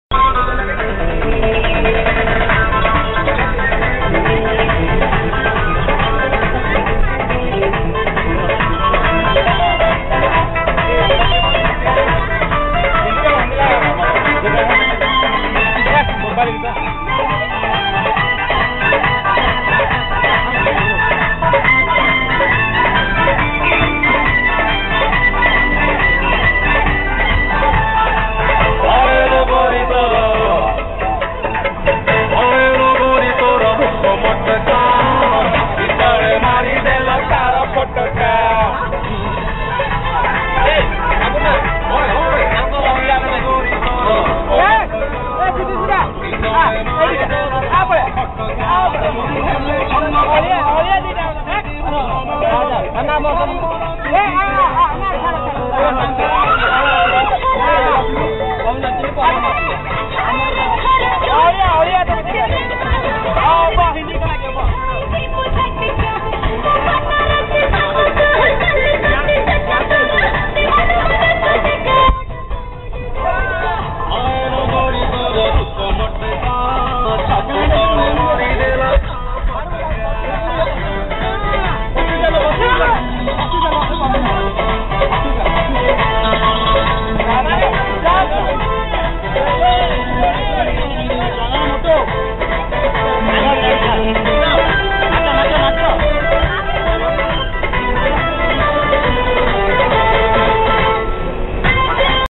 GHITM PICNIC PARTY OF ETC STUDENTS 1
THIS IS THE PICNIC PARTY OF GHITM 3RD YEAR ETC BRANCH (SECTION-B) STUDENTS HELD ON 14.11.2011 AT TARATARENI AND VAIRABI.OUR JOURNEY STARTED FROM PURI TO TARATARENI VIA VAIRABI.